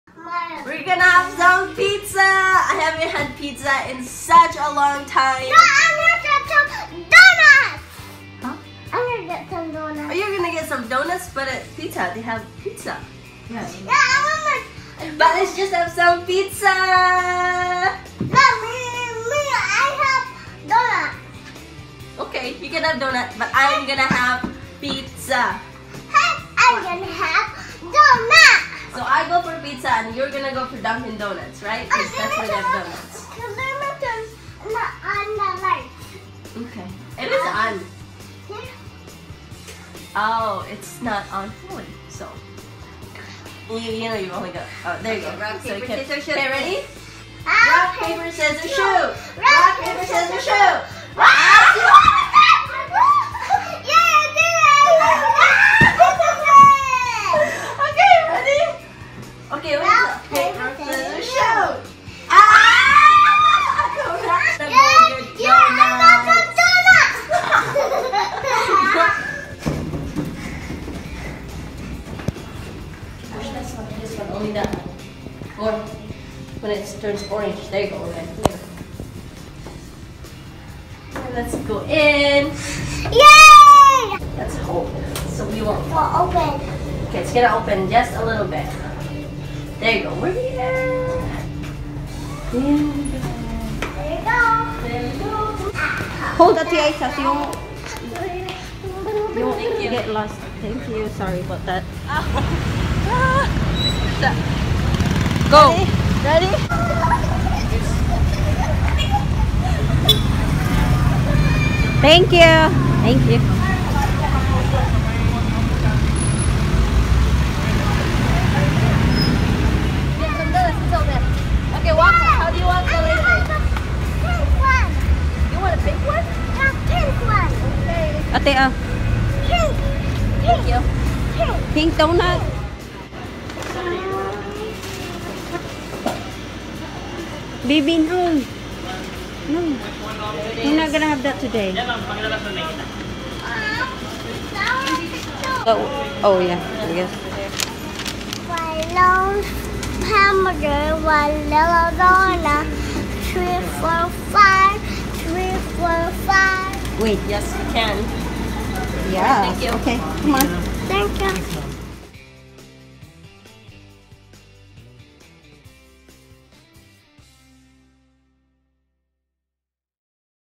Pizza or Donut fight for food sister vs. sister | Firebrand Girls
Who will win?. I want pizza. Adarah wants donut.